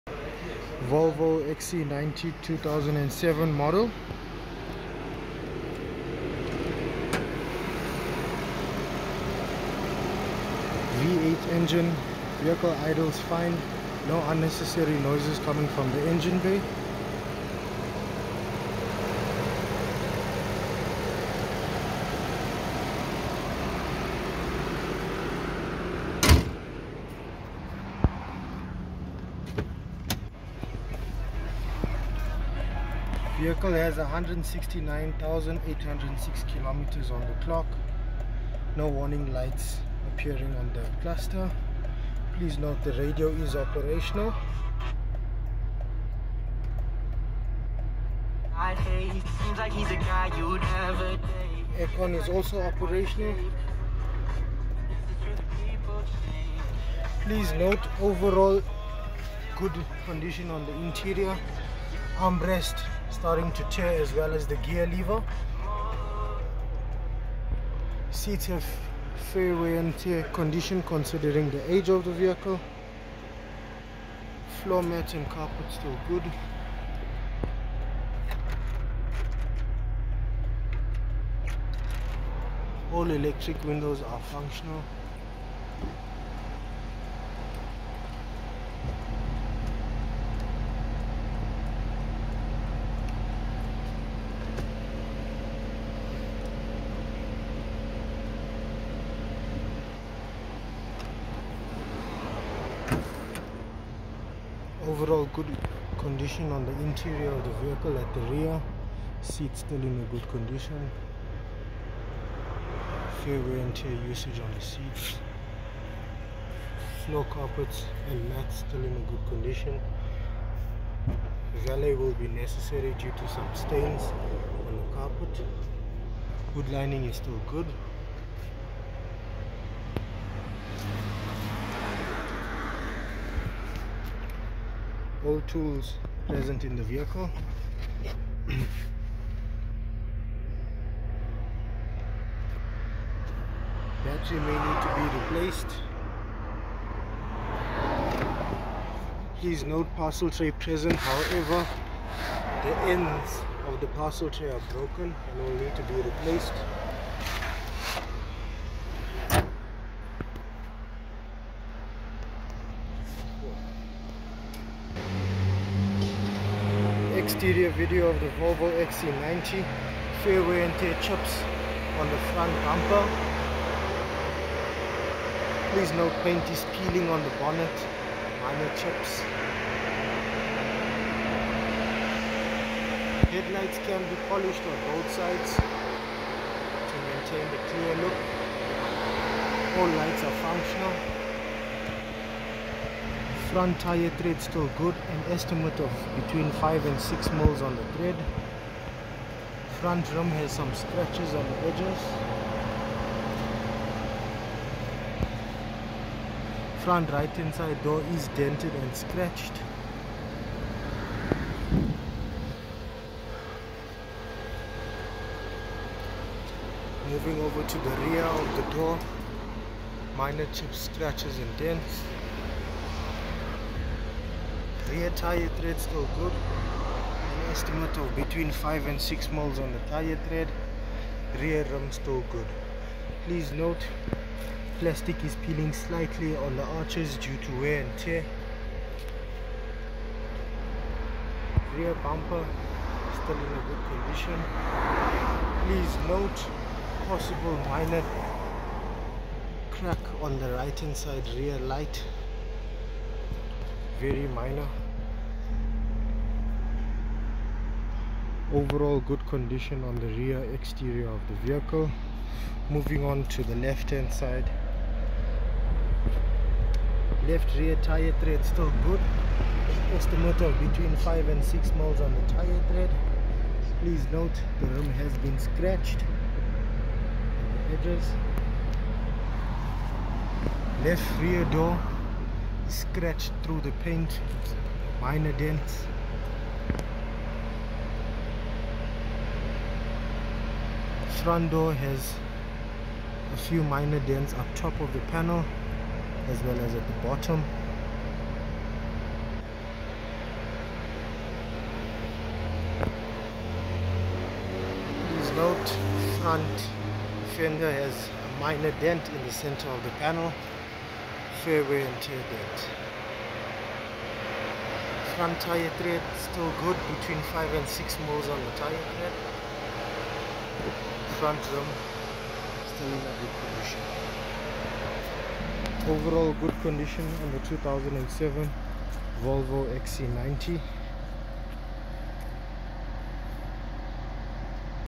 CarFling Inspection: 2007 Volvo Xc90 V8 Executive
Auction End Date: Thursday, 13 April 2023
Auction ID: 2166513

Vehicle: 2007 Volvo Xc90 V8 Executive
Mileage: 169 806km
Transmission: Automatic
Fuel: Petrol

Starting Bid: R65 000
Trade Price: R88 400
Reconditioning Cost: R10 800